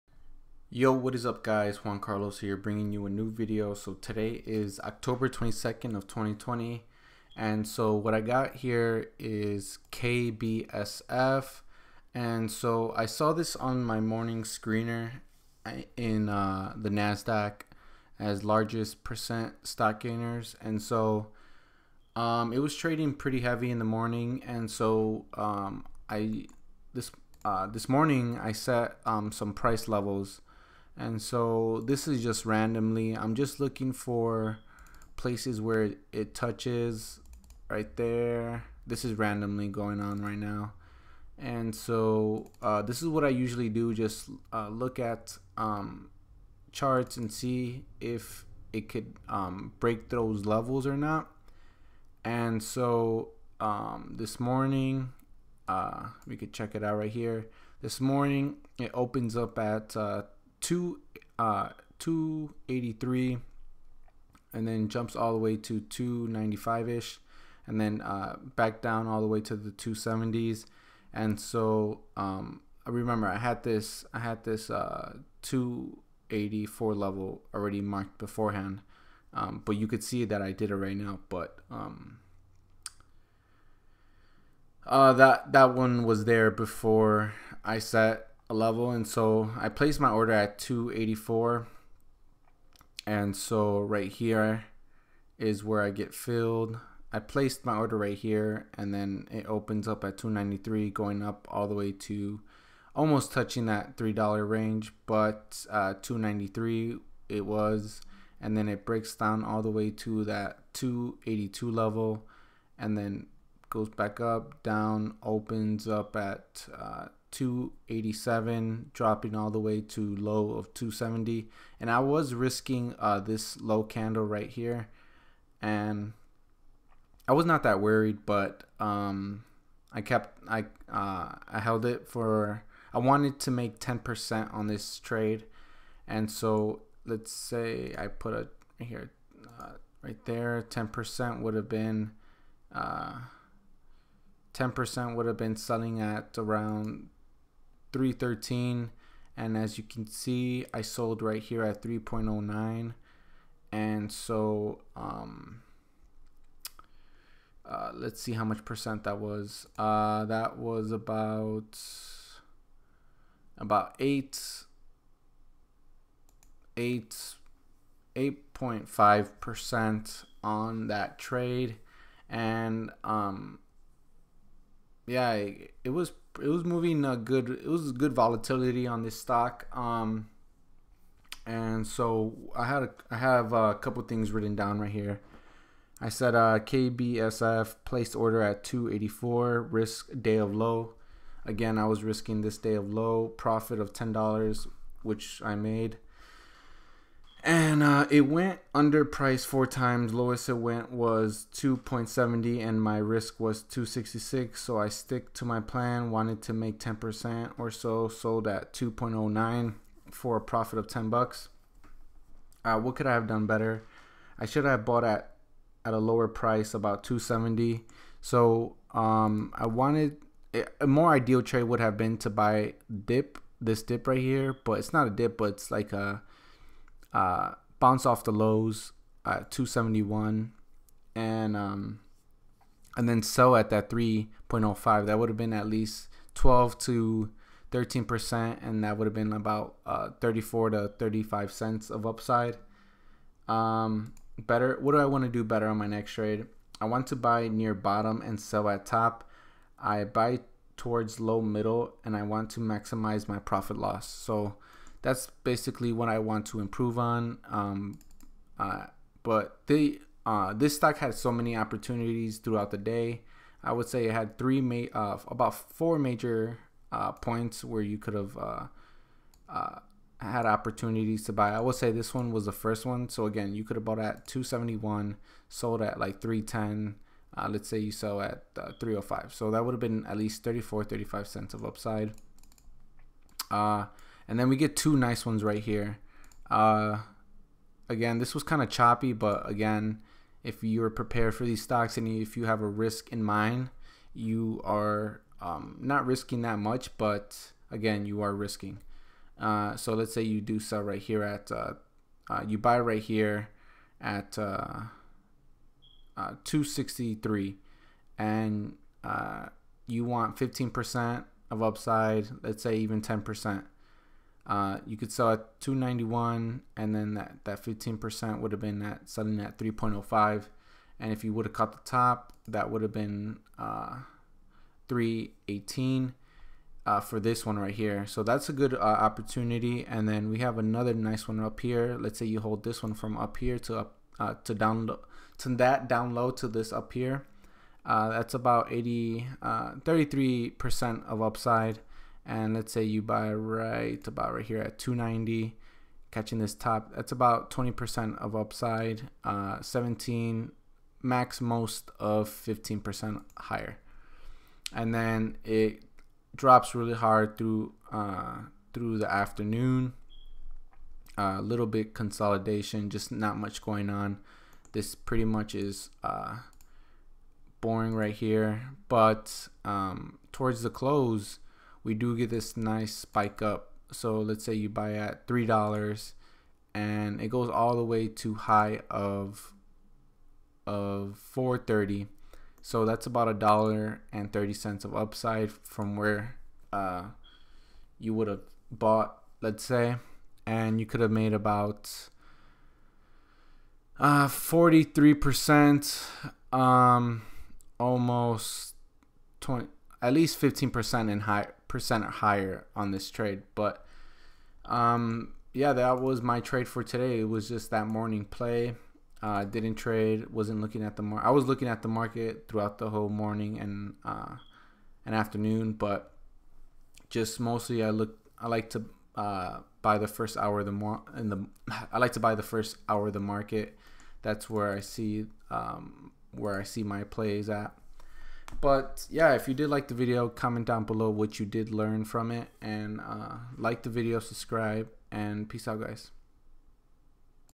KBSF - 158 - Look for Percentage
KBSF placed order @ 2.84
Risk Day of Low
Profit of $10.00
It went under price 4 times Lowest it went was 2.7 and my Risk was 2.66 so i stick to my plan wanted to make 10% or so sold @ 2.09 for a profit of $10
-What can i have done better?
I should have bought at a Lower price about 2.71 & sell @ 3.05 that would have been 30 cents of up side
-Better on next trade?
I want to buy Near Bottom and sell @ top I buy toward low middle & i want to maximize my P/L